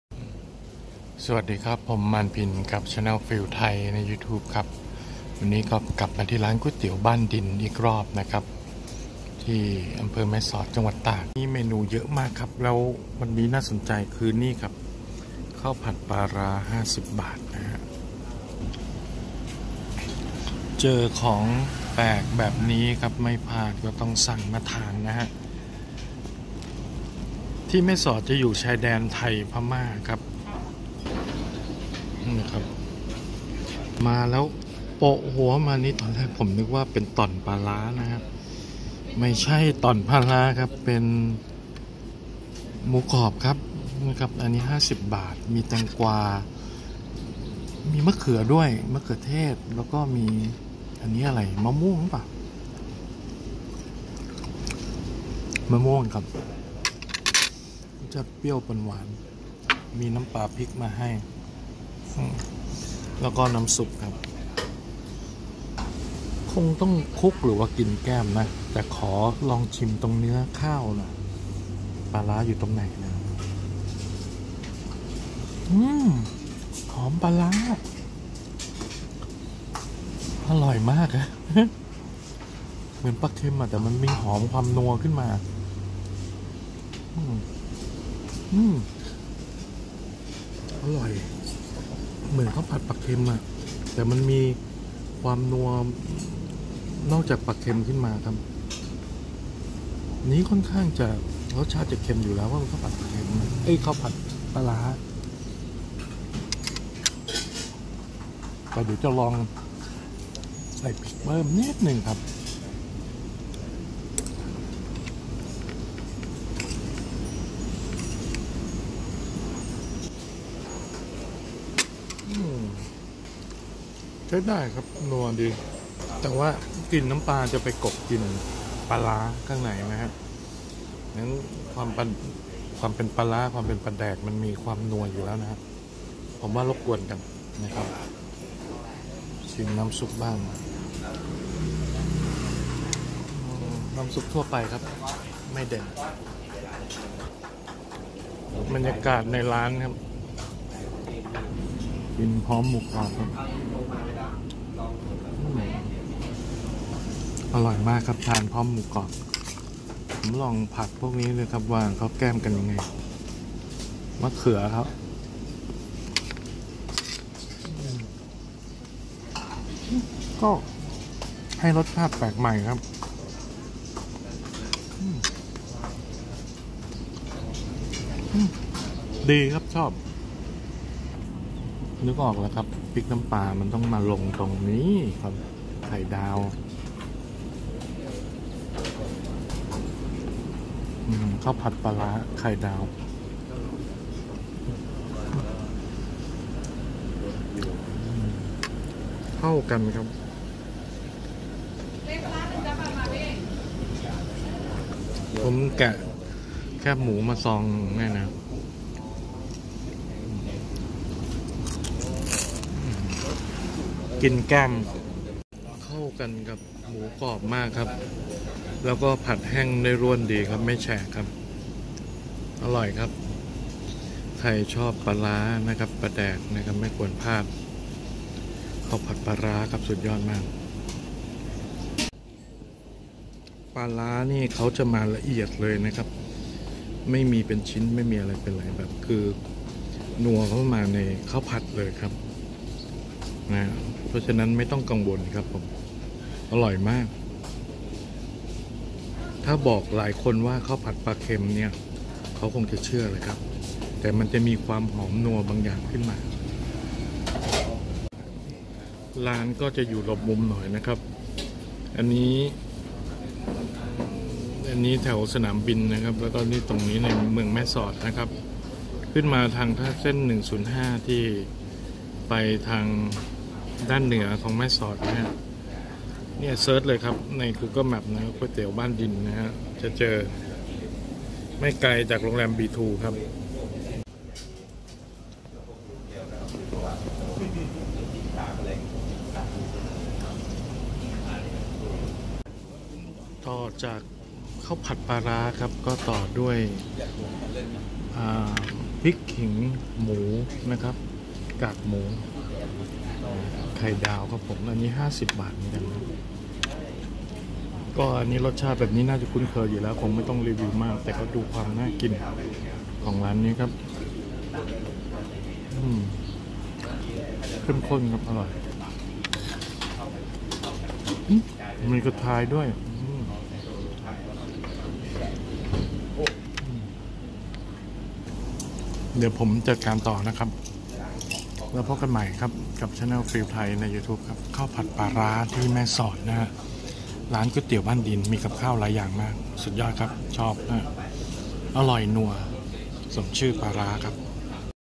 ข้าวผัดปลาร้าอร่อยมาก สูตรชายแดนไทย-พม่า
เมนูข้าวผัดปลาร้าแปลกดีเหมือนกันครับเป็นข้าวผัดกับปลาร้าอร่อยจริงๆคล้ายๆกับข้าวผัดปลาเค็มแต่จะมีความ อร่อยเป็นเอกลักษณ์ ขึ้นมาเพราะมีรสชาตินัวของปลาร้า เสิร์ฟพร้อมหมูกรอบทอด อร่อยเข้ากันมากครับ ทานที่ร้านก๋วยเตี๋ยวบ้านดิน อำเภอแม่สอดจังหวัดตากร้านนี้มีเมนูอะไรหลายอย่างน่าสนใจ